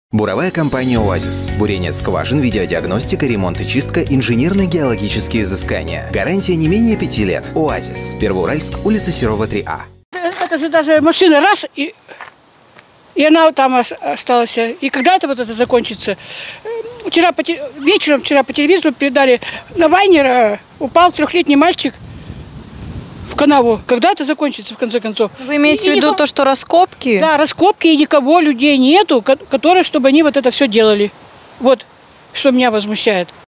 Раскопки на Вайнера
"Большая яма с водой и не огорожена"

Жители улицы Вайнера и Строителей возмущены большими раскопанными ямами, которые обтянуты сеткой.

В редакцию "Интерра ТВ" обратились горожане с просьбой обратить внимание на проблему раскопанной траншеи возле домов №13 "А" по улице Вайнера и дома № 14"А" по Строителей. Первоуральцы сообщили, что там очень опасное место, особенно в тёмное время суток. Раскопки обозначены хлипкой оранжевой сеткой, а освещения для такой большой ямы - нет. Кроме этого сама траншея частично заполнена водой, из воды торчат новые трубы. Люди пожаловались, что так недалеко и до несчастного случая.